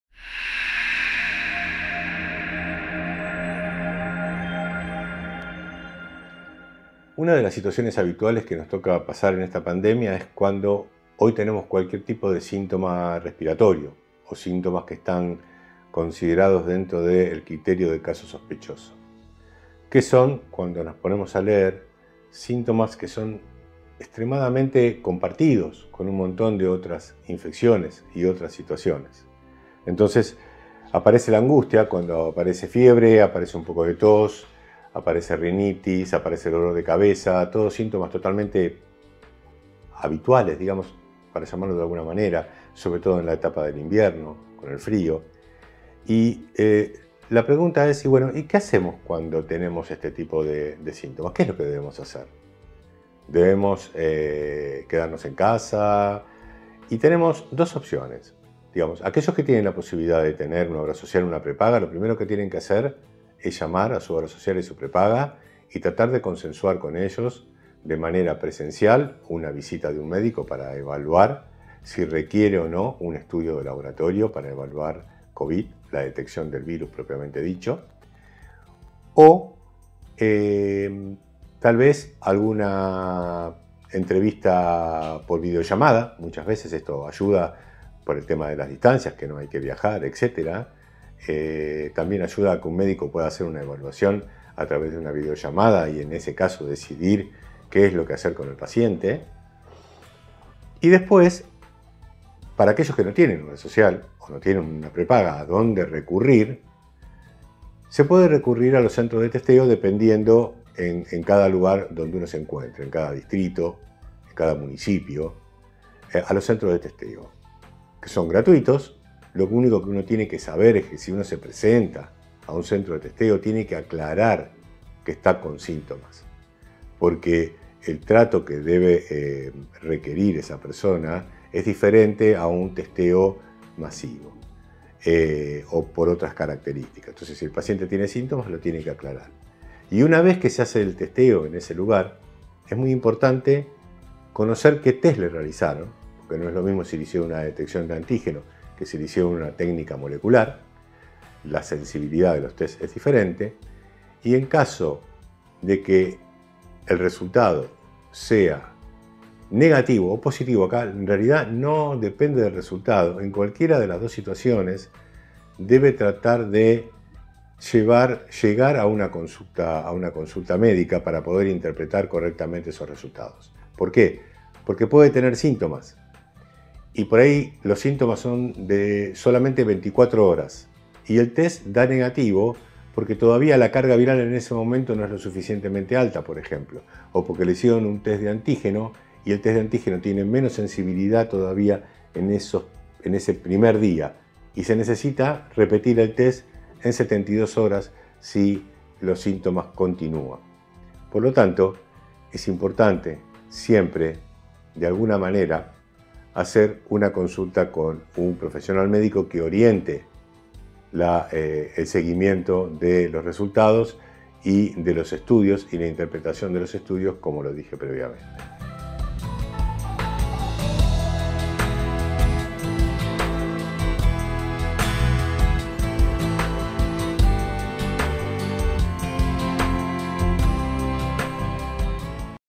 FUNCEIRESPONDE​ - Capítulo 50: Los test ante la presencia de síntomas compatibles de COVID19
Este es el capítulo 50 de una serie destinada a aportar conocimiento y contestar preguntas frecuentes sobre el Coronavirus, el cuadro infeccioso que produce y su situación epidemiológica.

En este video, el Dr. Edgardo Sturba, Bioquímico y microbiólogo Asesor científico de Stamboulian Laboratorio y FUNCEI, habla sobre Qué conducta debemos tomar ante la presencia de síntomas compatibles con la infección por SarsCov2​, el virus responsable del COVID19​ y qué profesional debería procesar las muestras.